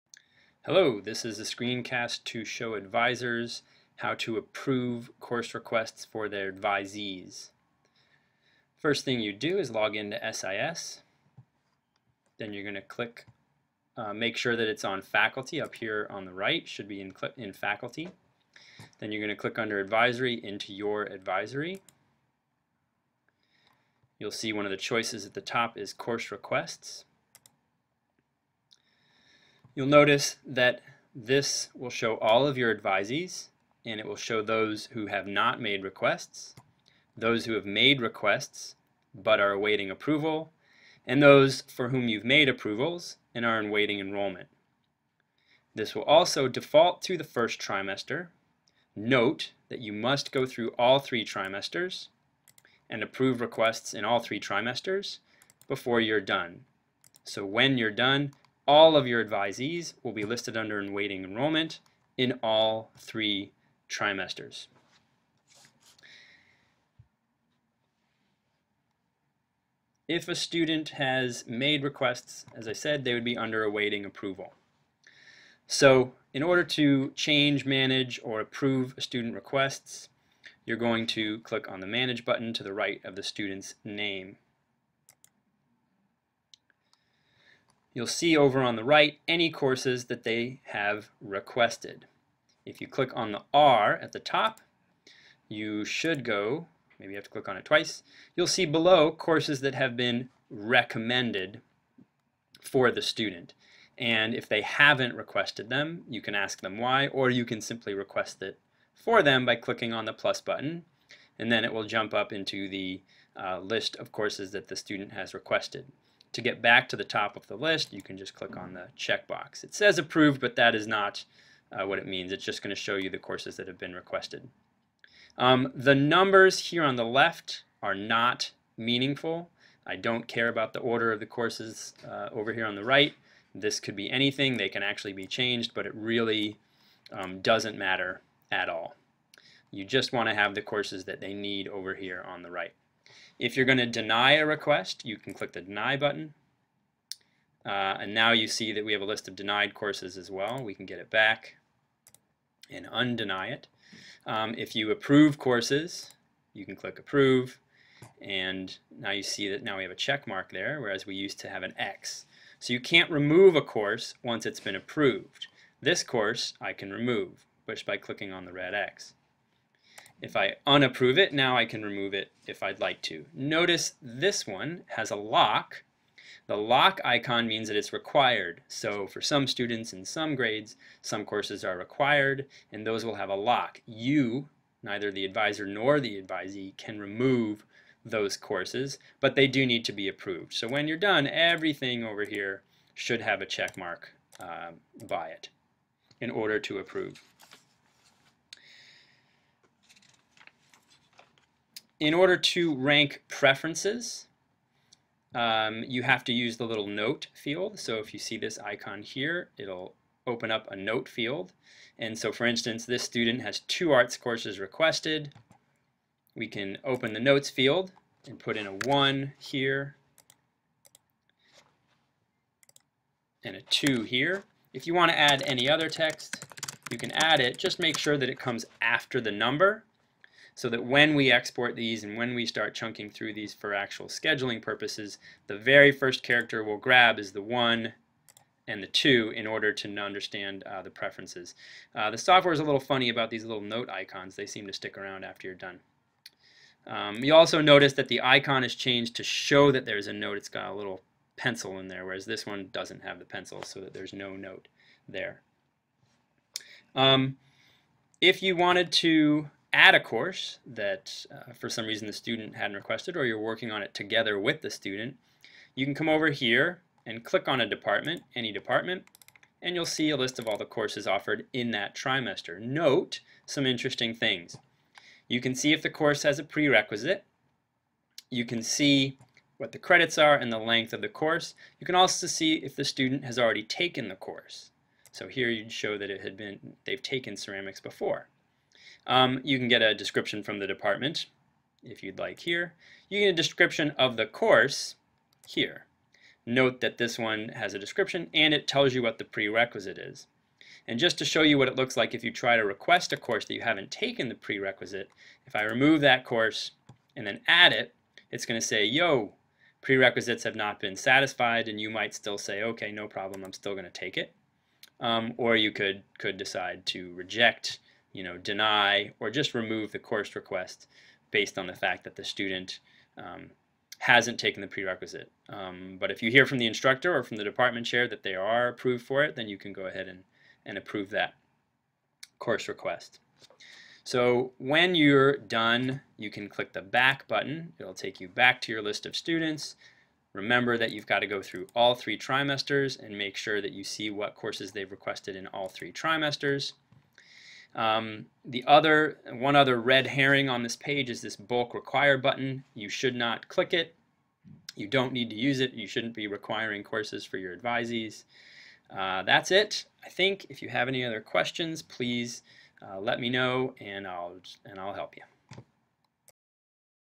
Course Requests Advisors - Old Pre 2017
Instructions for advisors to approve course requests. Edmund Burke School. Created 2014.